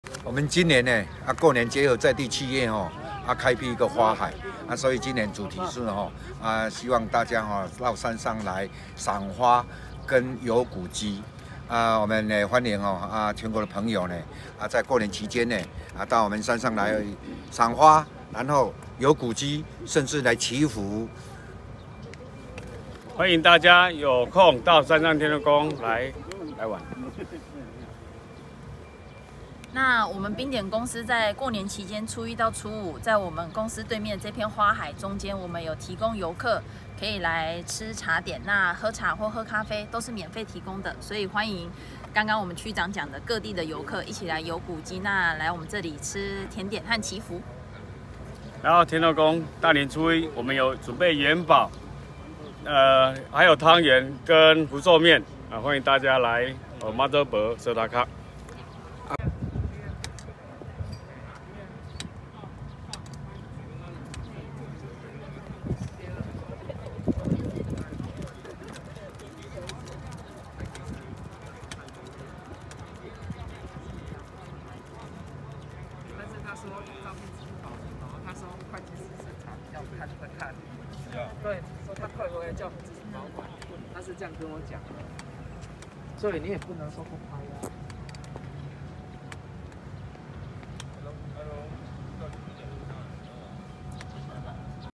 兔gether遊山上 彩色花海趣
記者張淑娟／山上報導

公私合作打造出山上美麗的一片片花海，今年山上區的迎春賞花季「兔gether遊山上」美呆了，將於二十二日至廿六日登場。區長余基吉表示，今年冰點空調冷氣提供土地遍植小百日草和波斯菊花海，新春到山上可「賞花趣、遊古蹟、佑祈福、好味道」。

走進山上區冰點花海，一排排彩色小風車迎風轉呀轉，轉出一片門五彩繽紛的花海，讓賞人心花開，余基吉表示，今年冰點負責人林正直提供場域，遍植花海，公所特以「賞花祈福、古蹟尋禮」為主題，結合蘭科植物園、花海區、山上花園水道博物館區及淨水池區花海，以絕美秘境花海，讓大家到山上來走春看花海，

花海祕境就在山上花園水道博物館-博物館區往淨水池區道路，是由冰點空調公司規畫種植百日草、波斯菊及油菜花，紅的、黃的、粉的、橘的美不勝收，現場還有充滿童趣的兔年主題裝置藝術，及一個大鞦韆；活動期間冰點特陳列名畫家Razor.H刻畫入微之作，還有免費咖啡、茶及限量董娘嚴選的Day day 私房手作甜點供遊客品嘗，藝術氣息滿點，視覺、味蕾也一次滿足。另初一至初三到現場打卡還可獲得公所提供的精美蘭花盆栽一株(每日限量五十株)及蘭科植物園折價券一張。

此外，山上天后宮主委蕭銘賢表示，大年初一特別準備限量新春五大禮--「人、車過七星平安橋祈福送山上黃金黑糖、廟中走春祈福送玉二媽ㄟ銀角仔鑰匙圈、樂捐者送玉二媽加持的平安米、初一憑山上花園水道博物館入場門票票根兌換玉二媽ㄟ銀角仔鑰匙圈或山上黃金黑糖、並備有甜湯圓、素食春麵、炸元寶與大家同歡，歡迎寺廟走春賀新年。